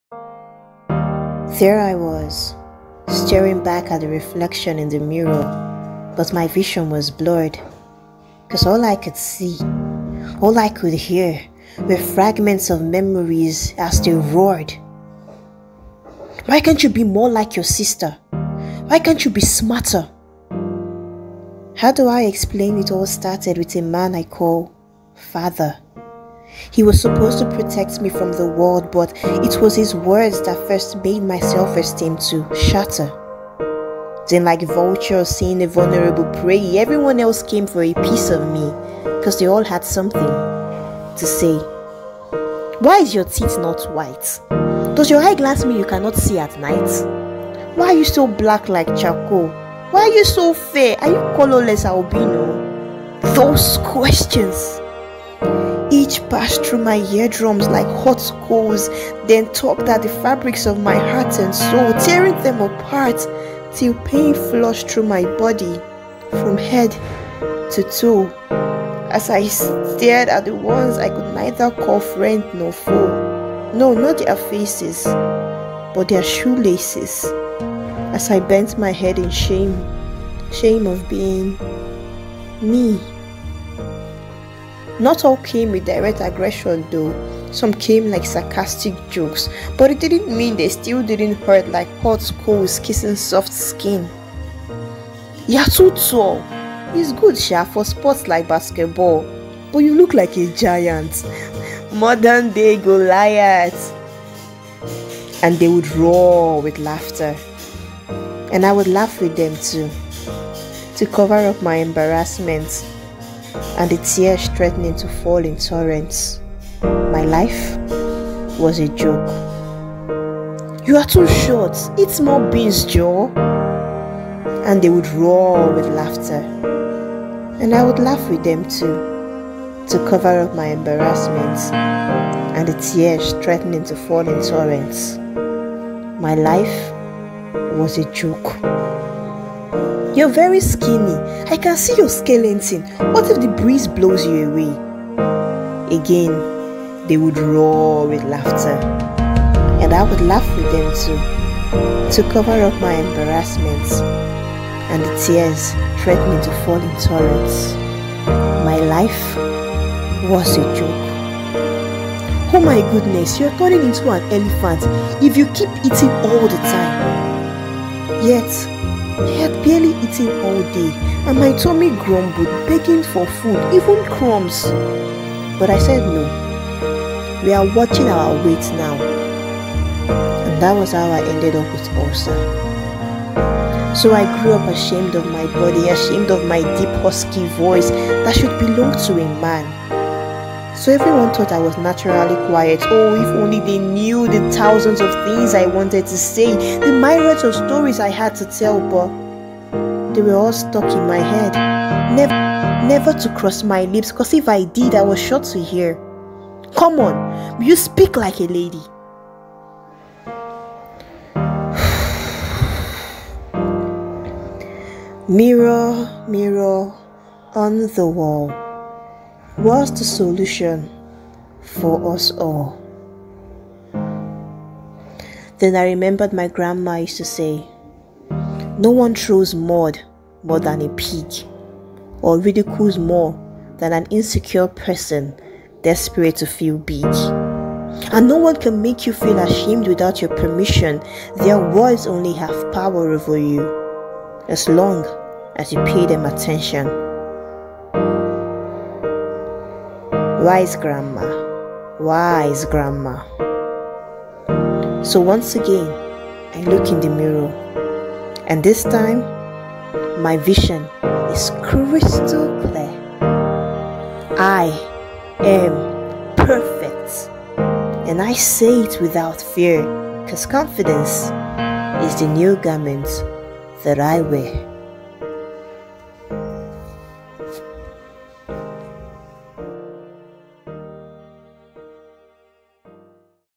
Mirror On The Wall (Spoken Word for SOC Anniversary)
The truth about the piece is that everyone of us can relate with it because at one point in life, we've heard some of these words or have been forced to laugh to painful jokes. At least,I have.

At a point, when working on this piece, I cried. Words hurt. We all need to be careful with things we say.

This is a spoken word piece on confidence, body shaming, low self esteem, mental health and self love. Be blessed.

Special thanks to John who makes 80% of my spoken word videos for putting this together on super short notice. I appreciate you beyond words. 💓💓